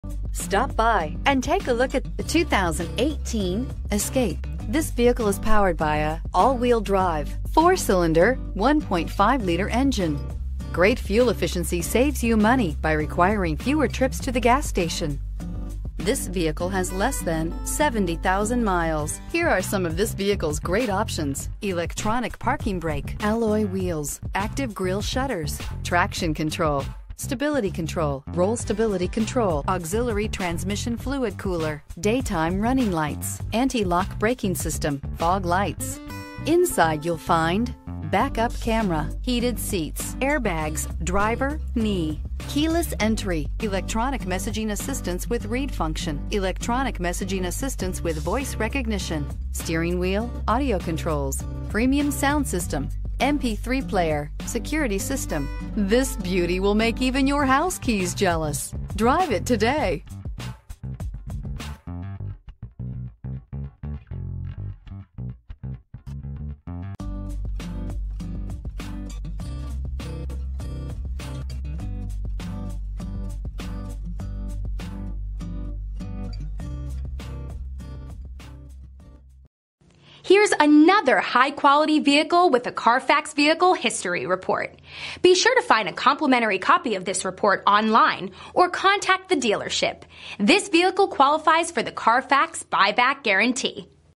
2018 Ford Escape Kingsport TN 6969P
2018 Ford Escape SE

2733 East Stone Dr.

Clean CARFAX.4WD. Blue 2018 Ford Escape SE 4WD 6-Speed Automatic 1.5L 4-Cylinder DGI Turbocharged DOHCRecent Arrival! 22/28 City/Highway MPGAwards:  * 2018 KBB.com Brand Image Awards   * 2018 KBB.com 10 Most Awarded BrandsPlease contact our internet department with any questions @ (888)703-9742.